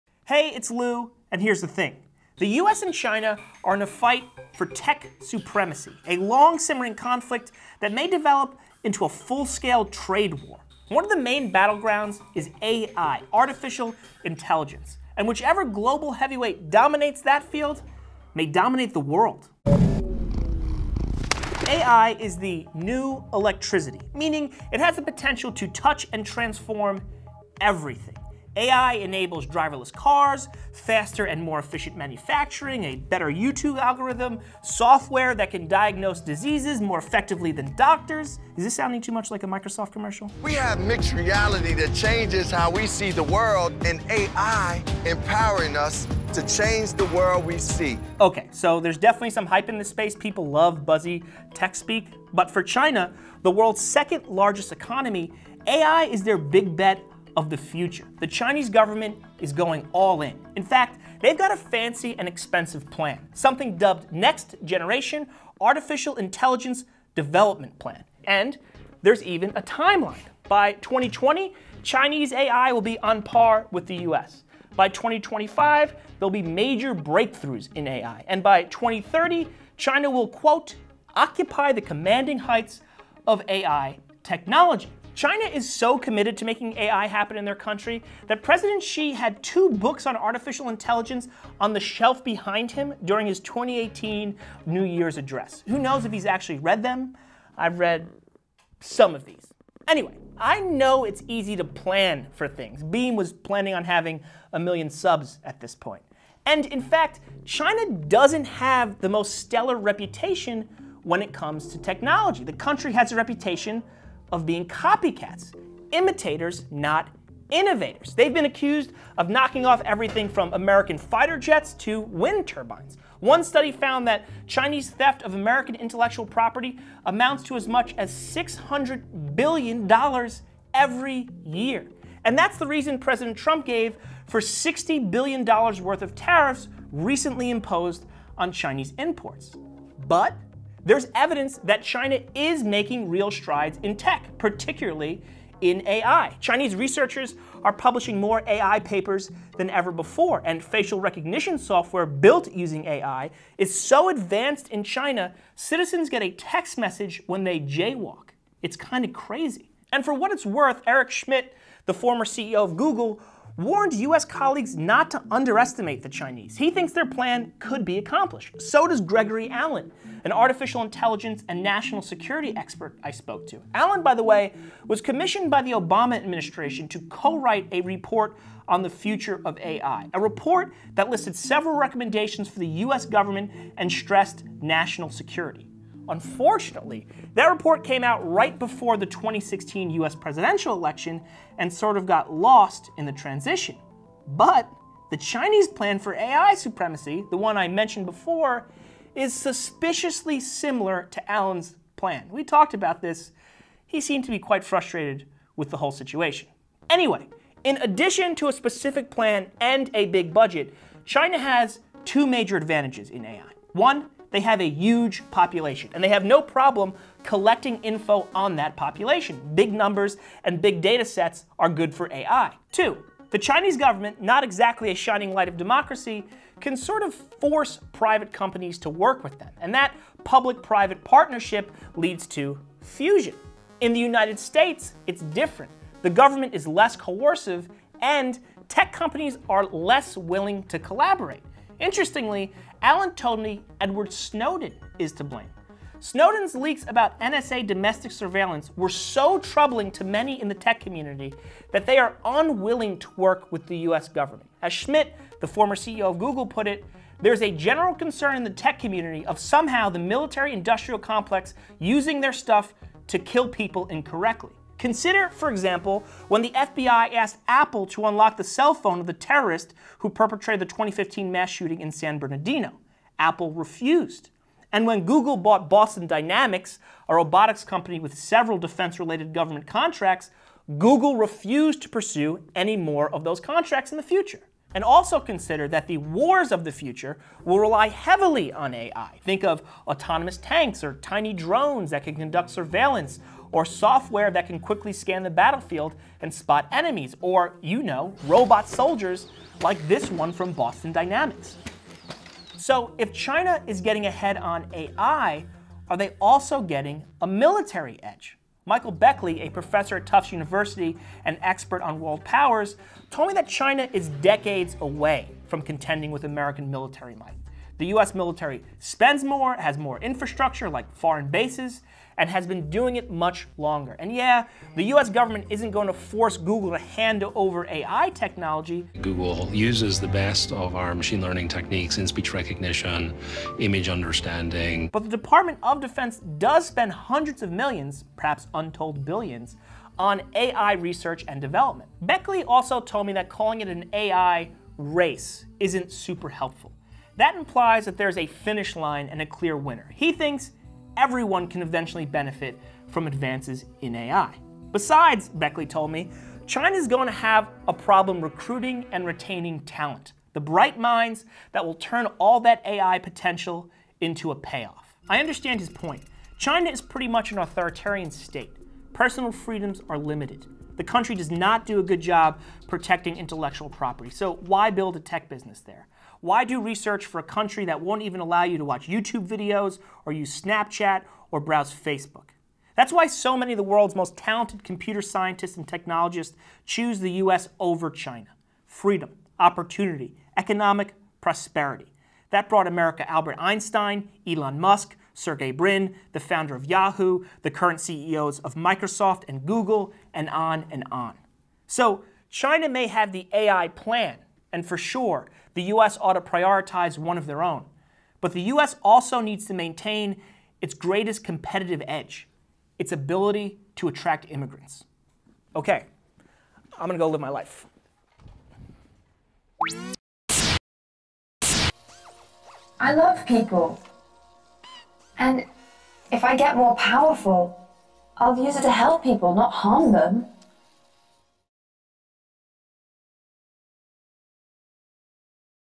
America's AI War with China
A US-China trade war might be on the horizon, but a darker technological war has already been brewing - the fight over Artificial Intelligence. Lou looks into how both the US and China have been advancing their own AI, and the deeper implications of this new-age tech war.

Sources & Further Reading:

As China Marches Forward on A.I., the White House Is Silent

China’s Plan to ‘Lead’ in AI: Purpose, Prospects, and Problems

Chinese theft of sensitive US military technology is still a 'huge problem,' says defense analyst

“Artificial Intelligence and National Security”, Greg Allen & Taniel Chan (pdf)

Google Employees “Outraged” Their Tech Is Being Used To Build Better Killing Machines

Tech As We Know It Would Not Exist Without Immigrants

CREDITS
Writer: Louis Foglia
Editor: m.cho
Researcher: Dushyant Naresh